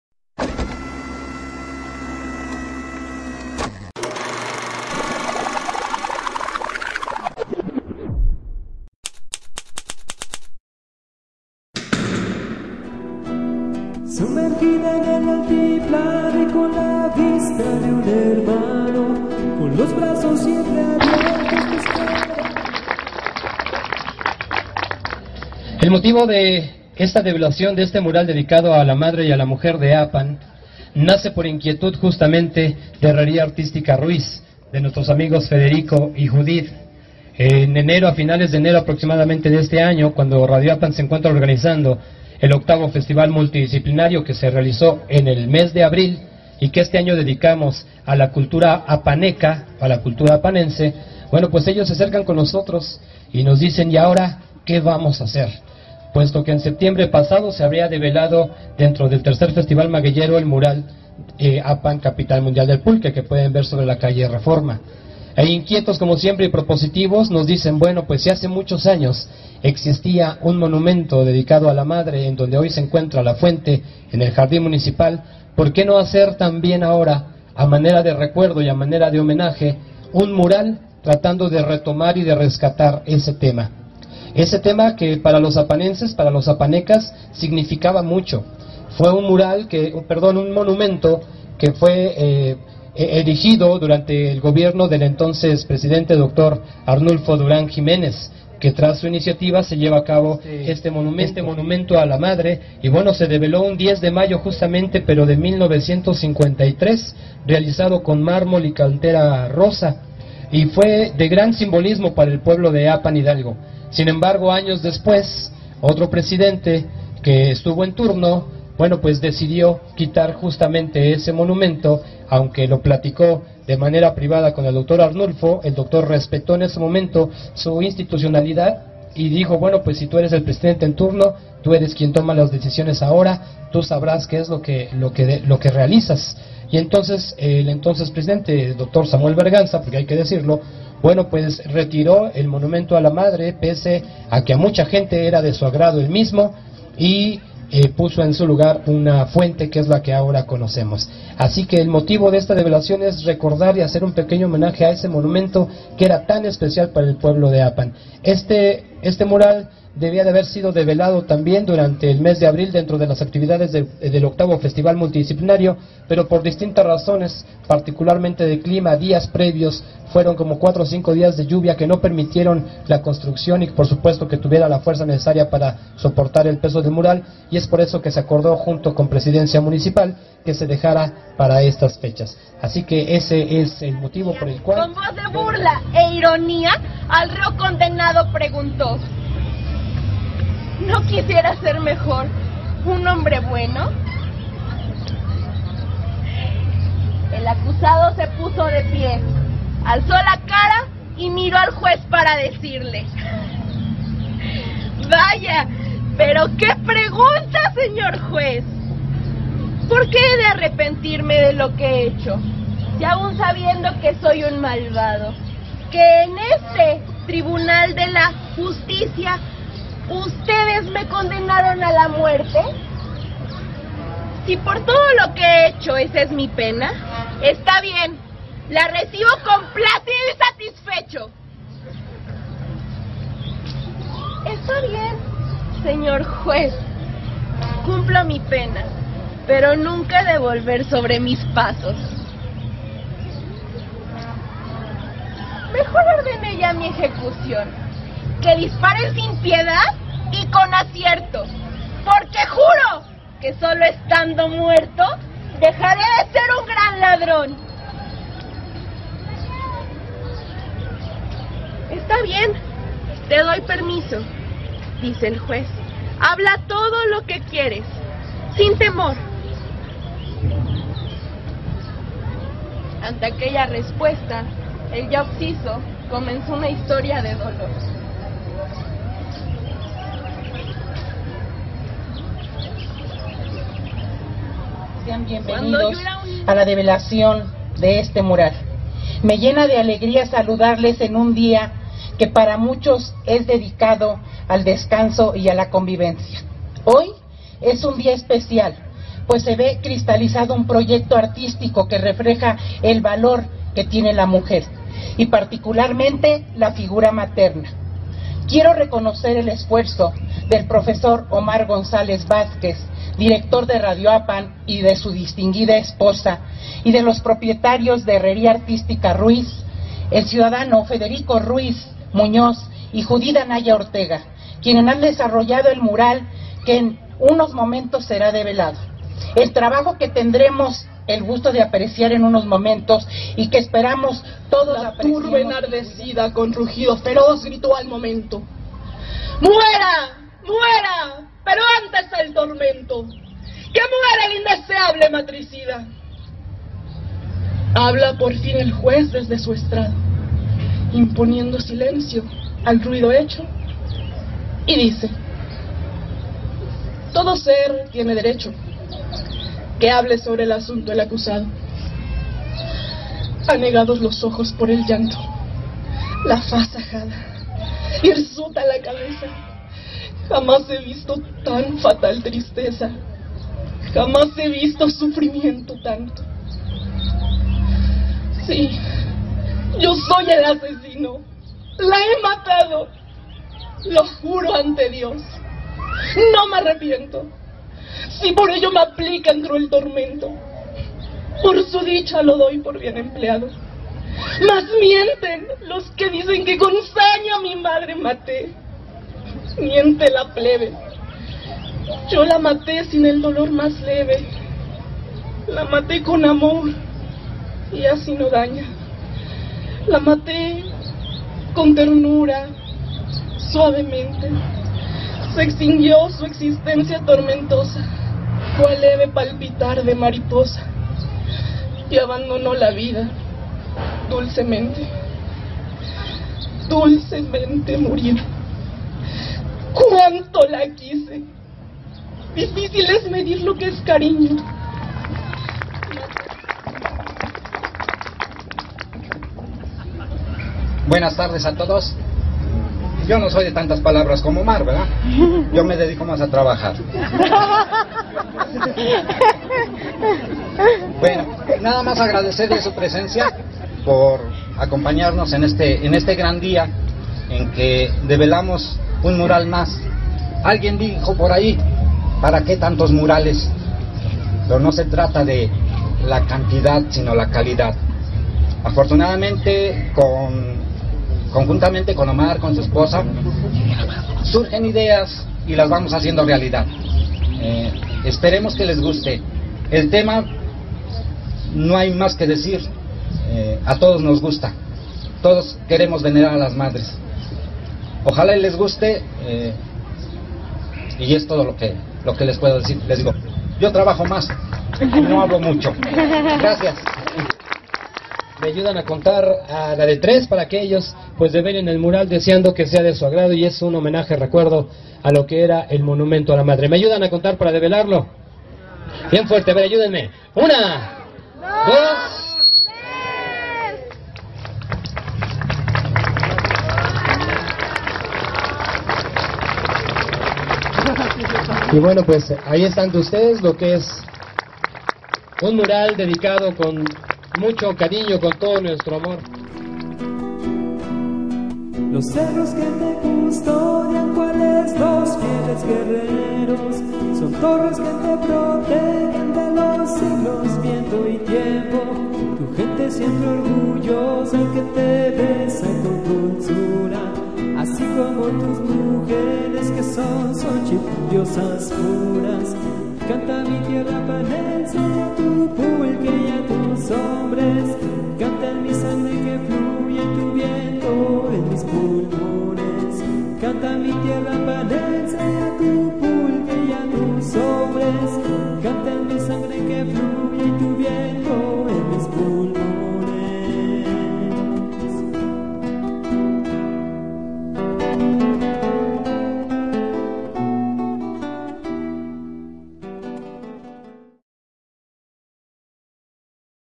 Develan Mural a la Madre y Mujer de Apan
Mural a la Madre y Mujer de Apan fue develado el Domingo 7 de Mayo del 2017.